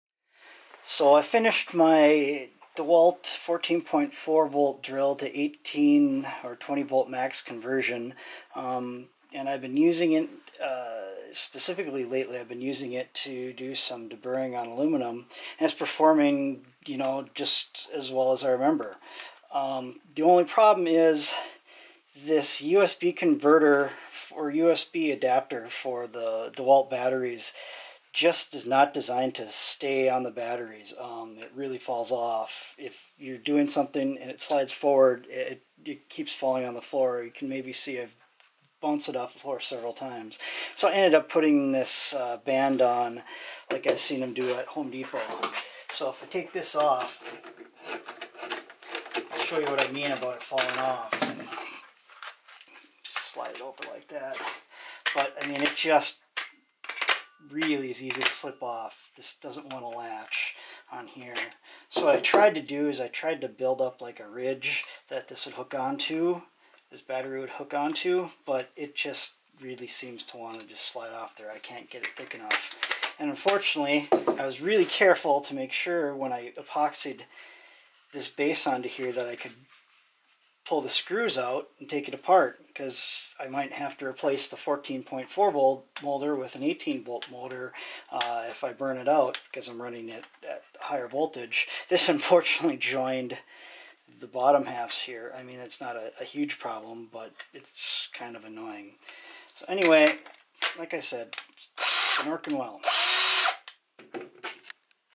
DeWalt DC983 14.4V Drill Converted to 20V Max
I converted my Dewalt DC983 14.4V NiCad Drill to use a 20V Max Li-Ion battery.

Sorry I'm a little off center in this video.